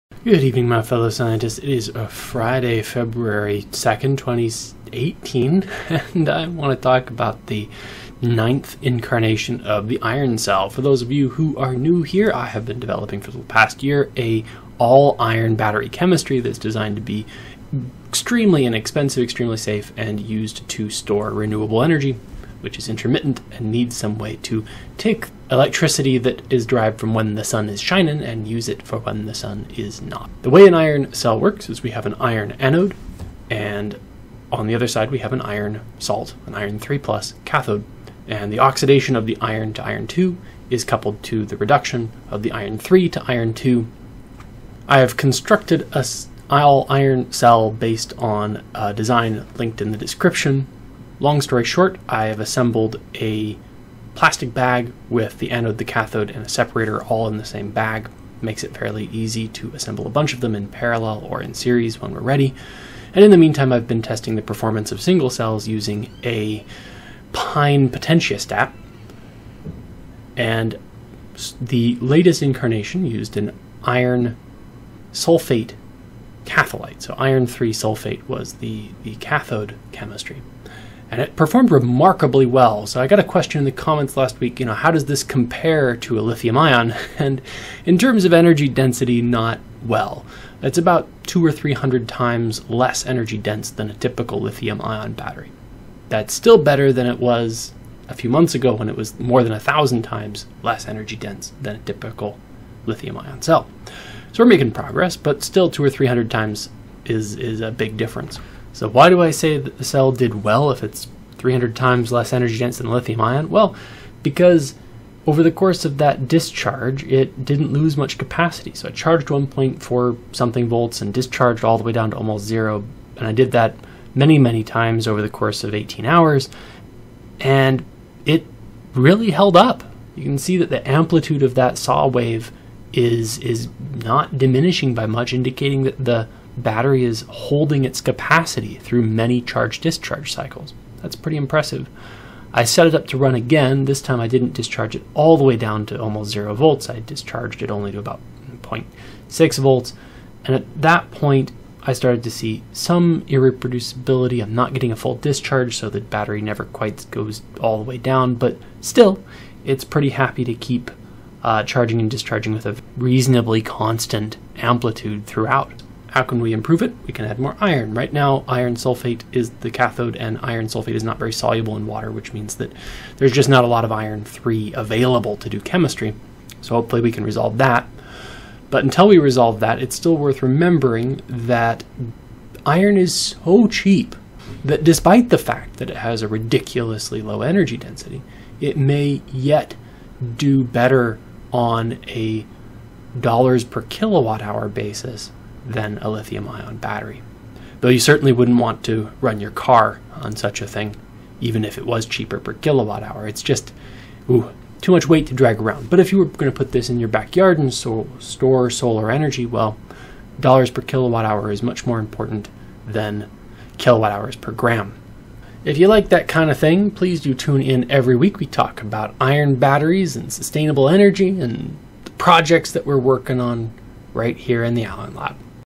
Iron Battery part 60: 24 Deep Charge-discharge Cycles
This time I ran 35+ hours of charge-discharge cycles on the iron sulfate cell and it looked pretty stable.